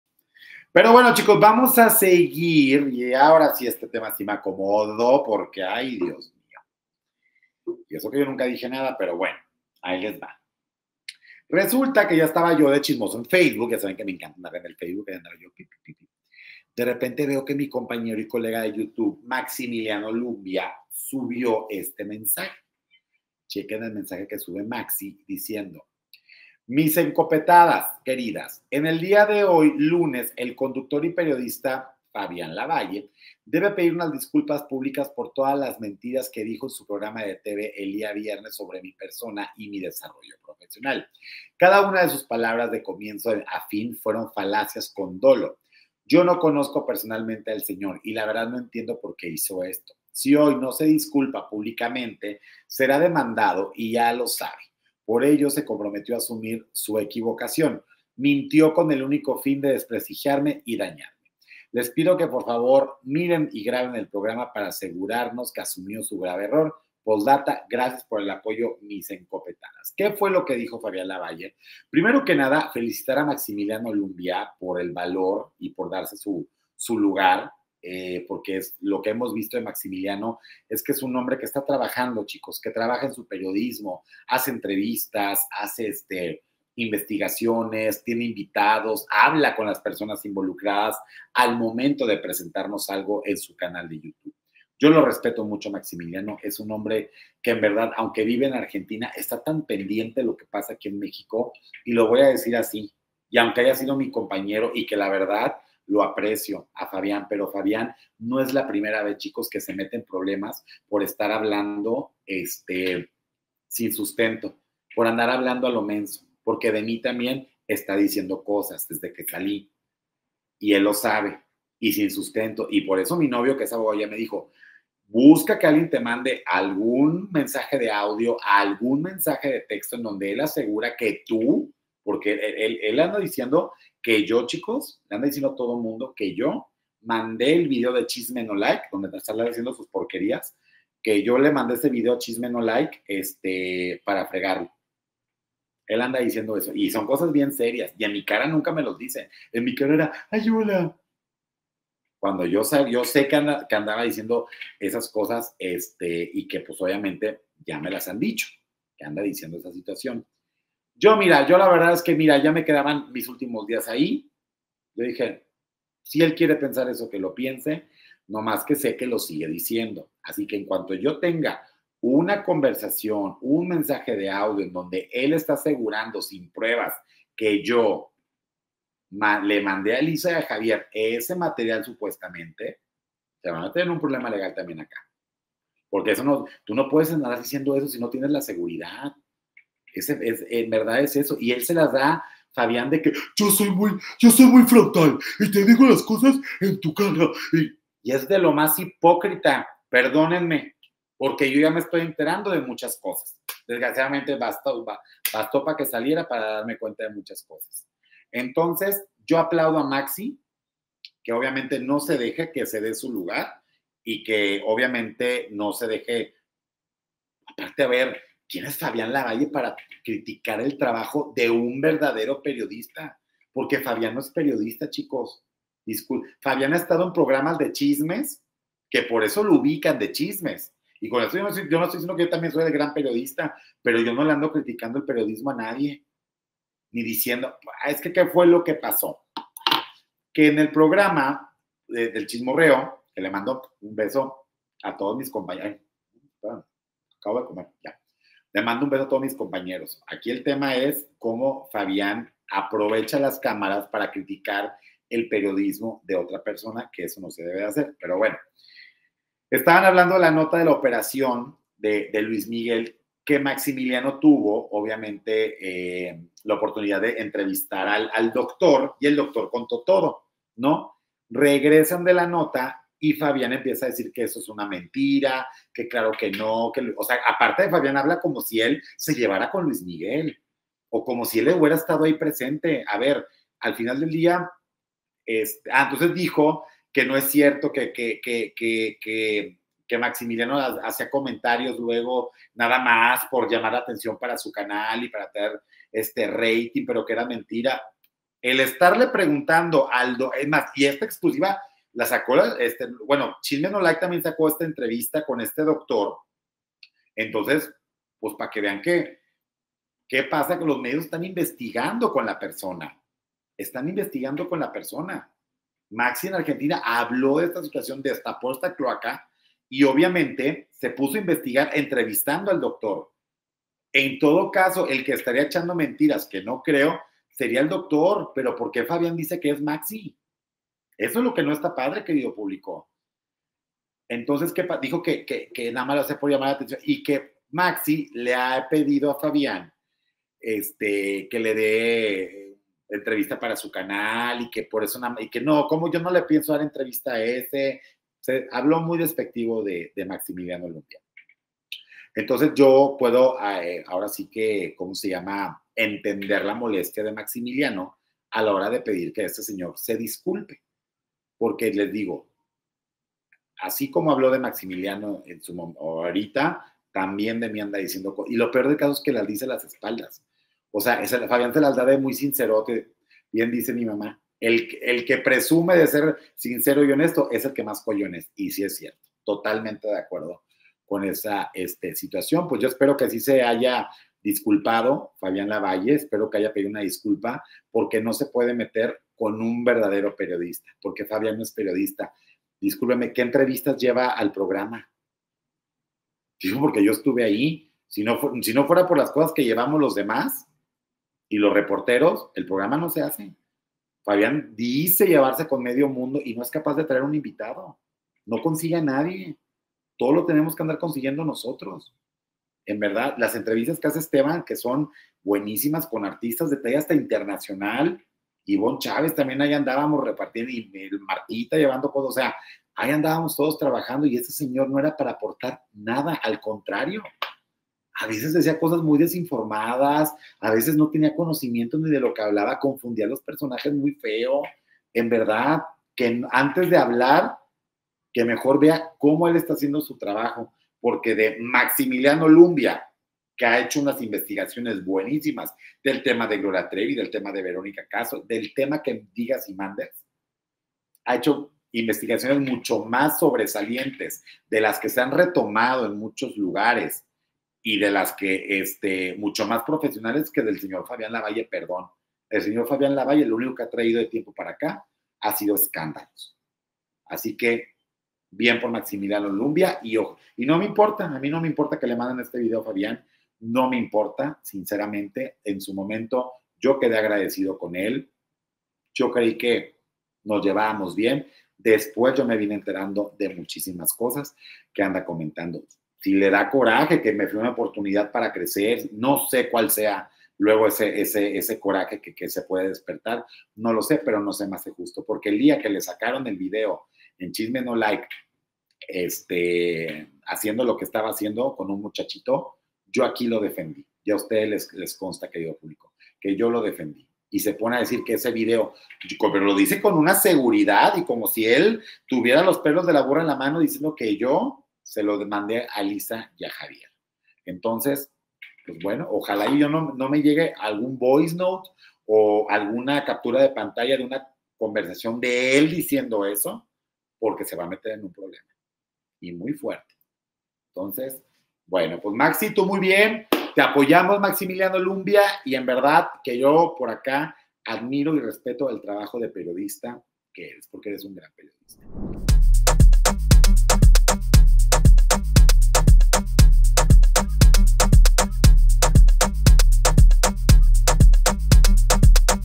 Maximiliano Lumbia exige que Fabián Lavalle se retracte y pida disculpas | MICHISMESITO
MaximilianoLumbia exige que FabiánLavalle se retracte y pida disculpas